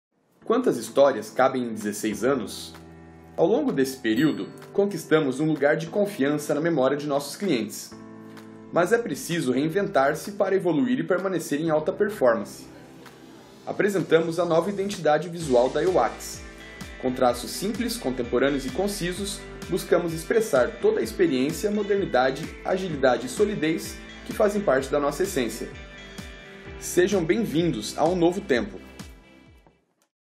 Quantas histórias cabem em 16 anos? | Euax Consulting
A Euax completa 16 anos de muita experiência, agilidade e modernidade. E para traduzir tudo isso, lançamos uma nova identidade visual!

Sejam bem-vindos a um novo tempo!